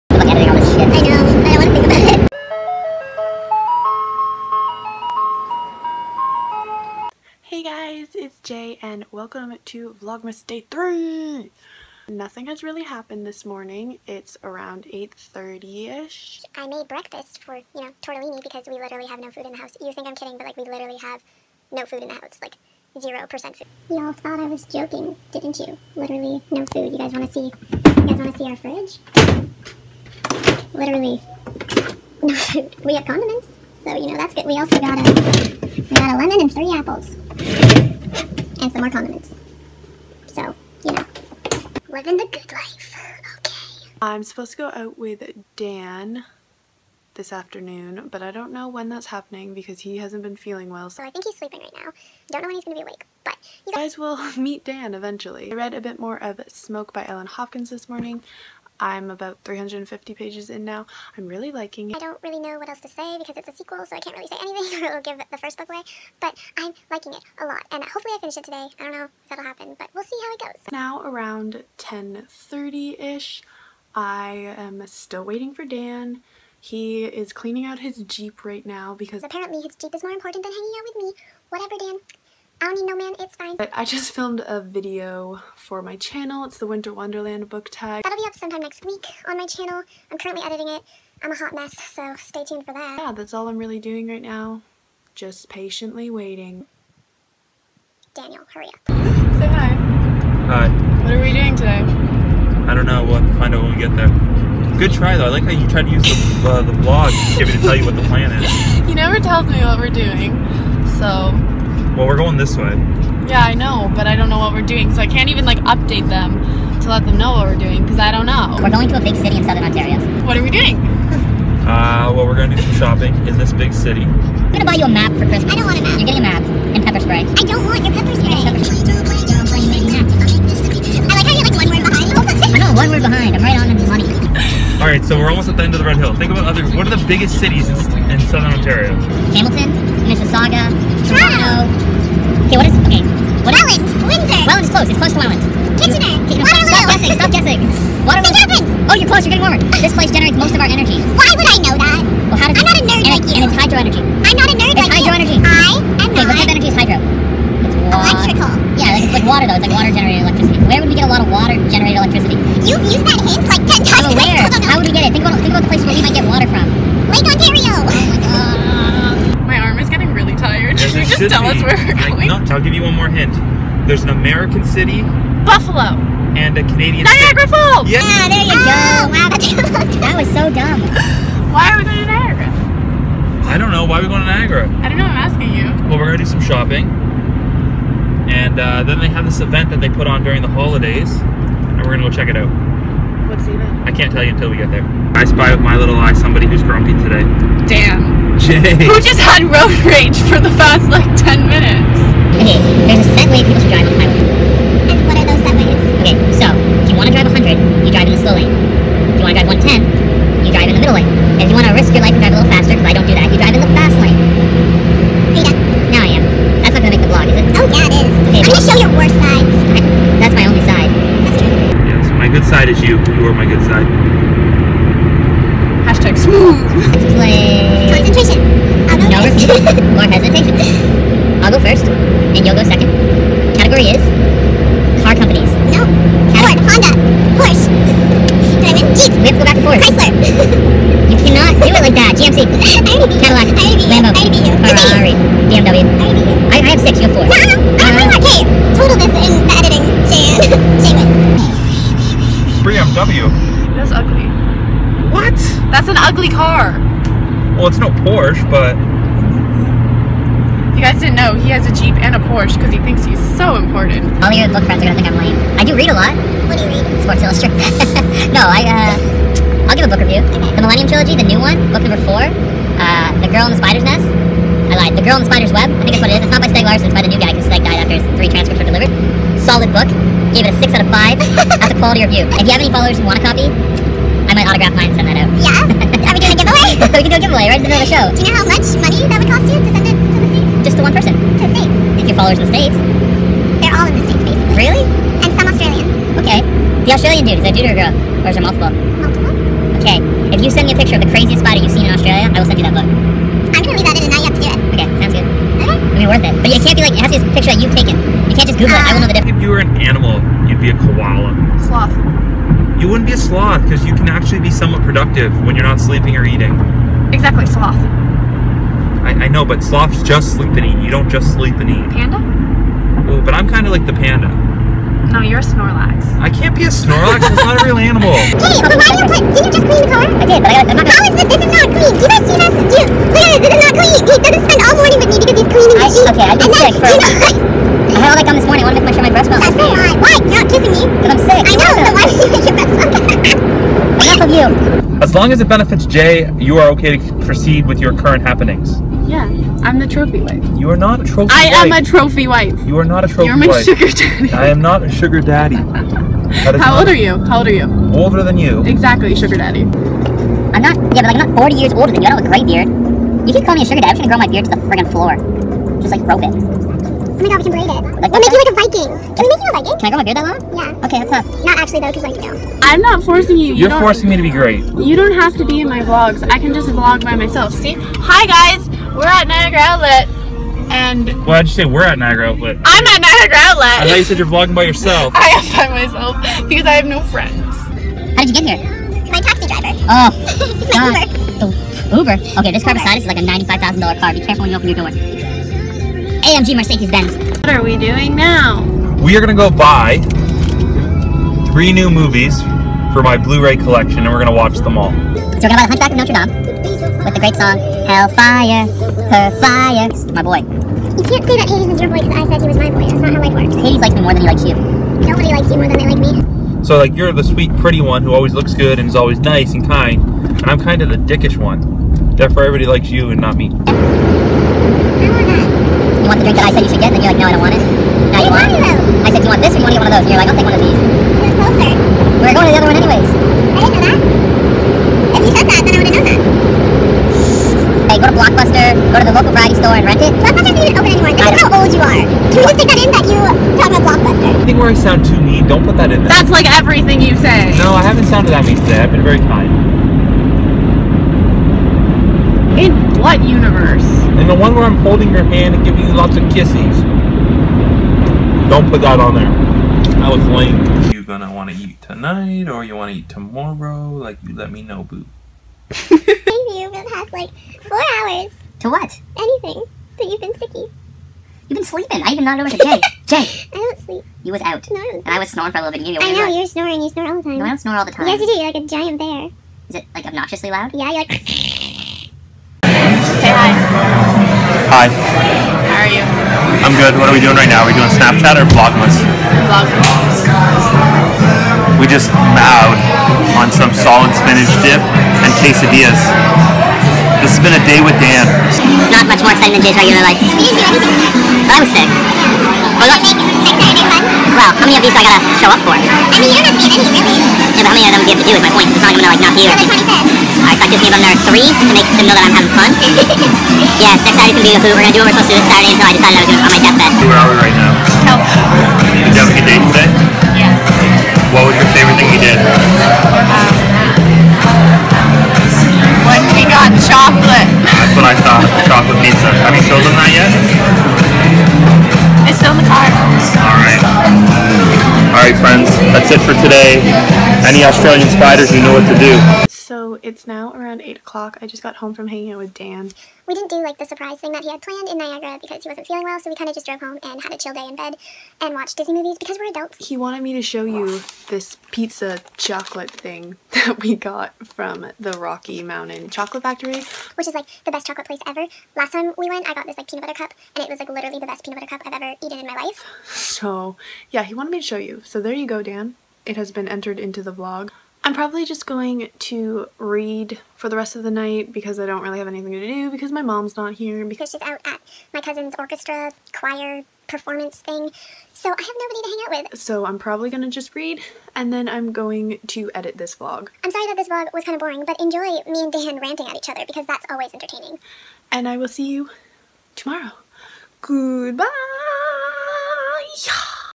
The Roast of Dan | Vlogmas 2016 - Day 3
Oh look, 2 videos in one day, I'm not sorry.
Watch 10 minutes of Jay and Dan roasting each other because that's what couples do ;)